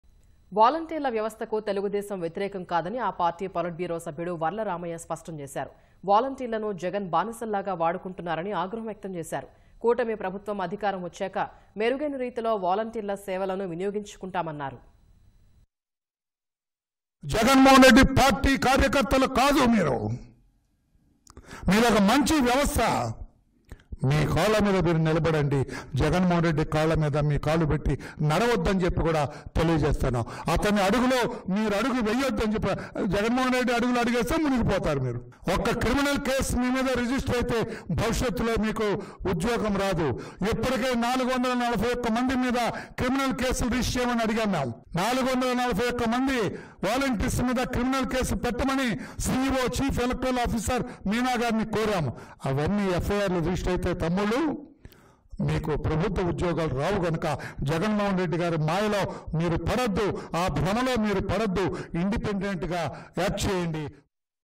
వాలంటీర్ల వ్యవస్థకు తెదేపా వ్యతిరేకం కాదు | TDP Not Against Volunteer System | Varla Ramaiah
వాలంటీర్ల వ్యవస్థకు తెలుగుదేశం వ్యతిరేకం కాదని ఆ పార్టీ పొలిట్ బ్యూరో సభ్యుడు వర్ల రామయ్య స్పష్టం చేశారు. వాలంటీర్లను జగన్  బానిసల్లాగా వాడుకుంటున్నారని ఆగ్రహం వ్యక్తం చేశారు. కూటమి ప్రభుత్వం అధికారం వచ్చాక మెరుగైన రీతిలో వాలంటీర్ల సేవలను వినియోగించుకుంటామన్నారు.